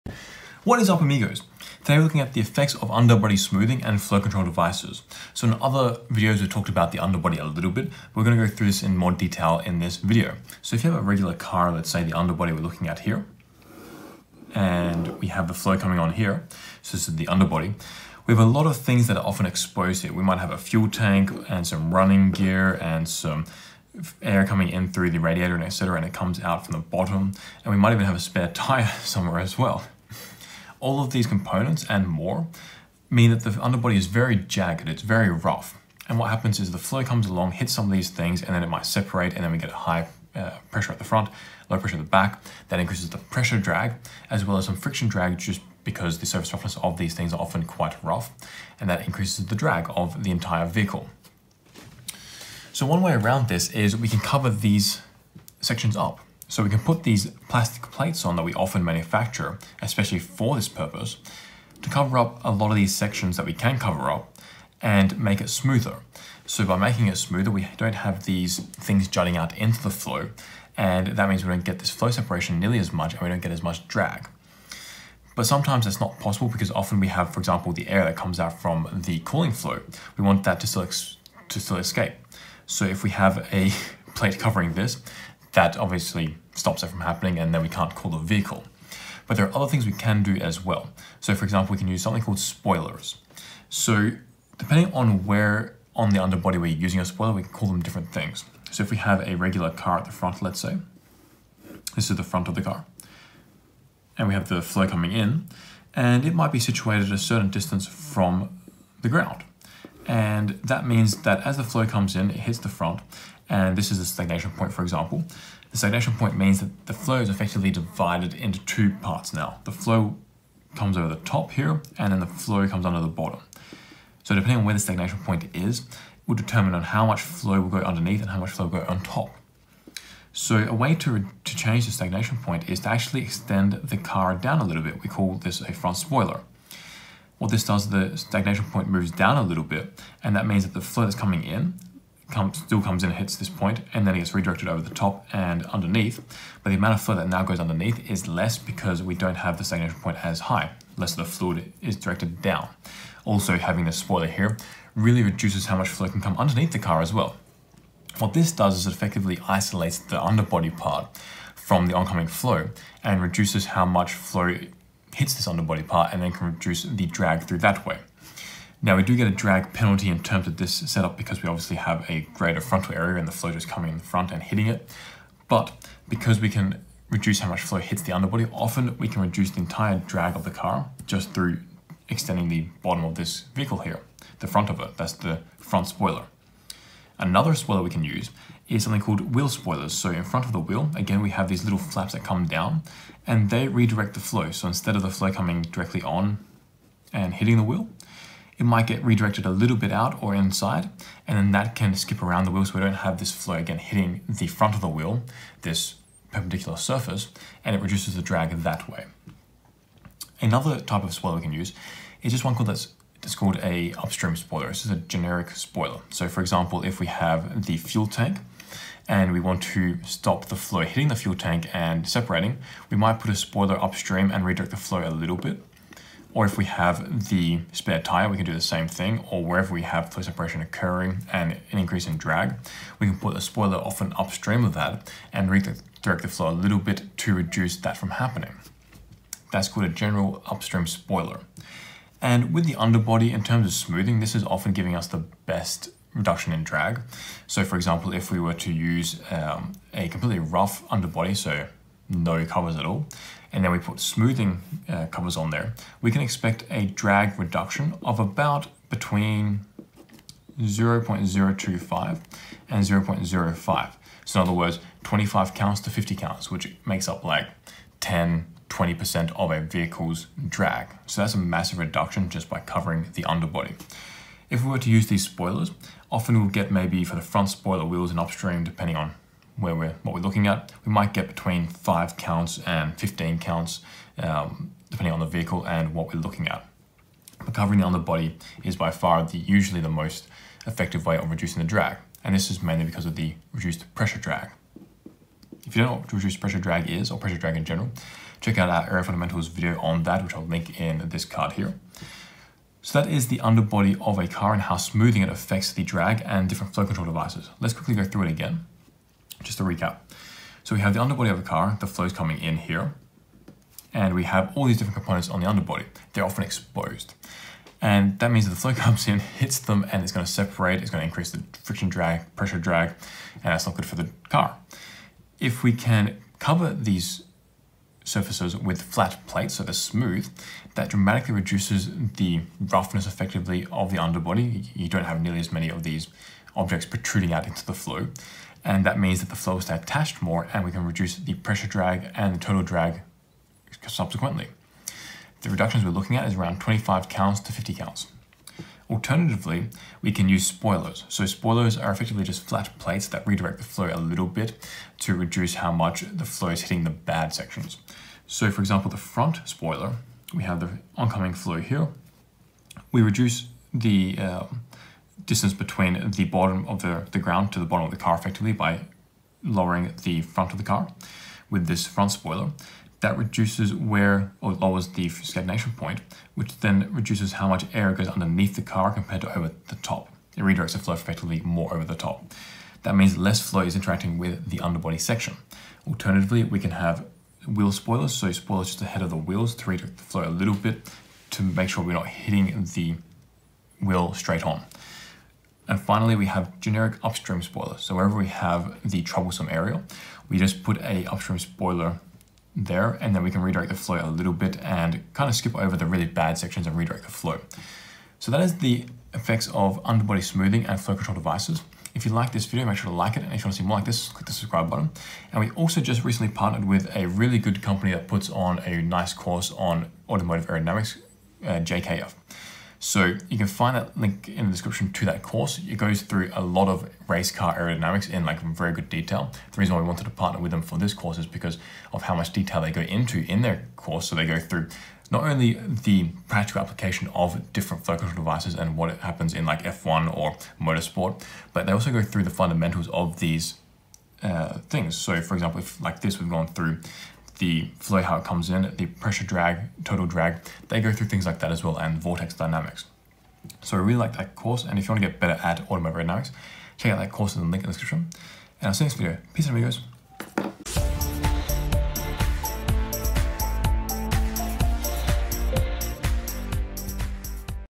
{Automotive Aero #20} Underbody Smoothing and Flow Control Devices
A car's underbody is often exposed and creates a lot of flow separation and pressure drag. How can you make the underbody of a car produce less drag?

This video explains this.